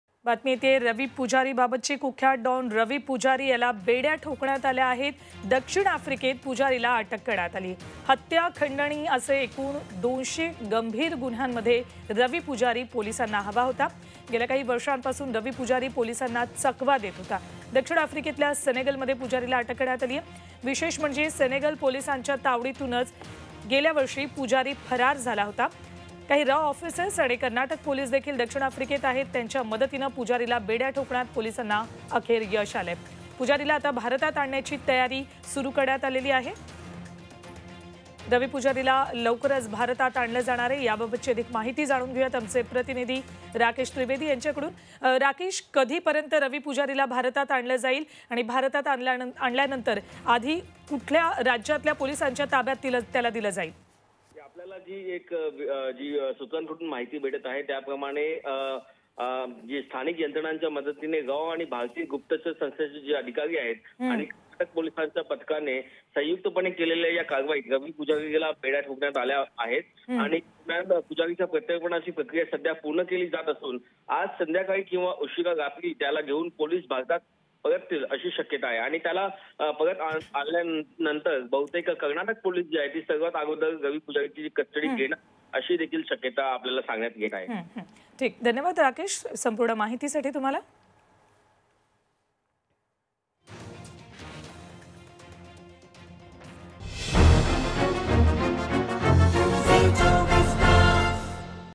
अंडरवर्ल्ड डॉन रवी पुजारीला बेडया
Underworld Don And Gangster Ravi Pujari Detained In South Africa